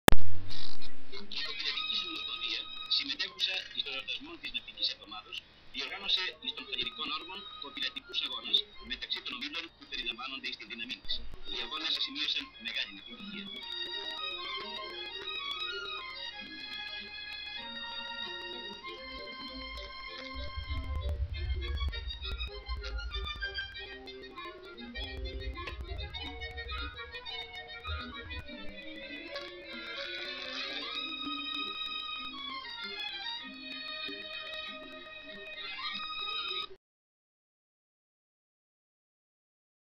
Κωπηλατικοί αγώνες στο Φαληρικό Όρμο
Κωπηλάτες μεταφέρουν λέμβους στην παραλία του Φαληρικού Όρμου, όπου διοργανώνονται αγώνες από την Κωπηλατική Συνομοσπονδία για τον εορτασμό της Ναυτικής Εβδομάδας. Πληρώματα κωπηλατικών λέμβων λαμβάνουν μέρος σε αγώνα κωπηλασίας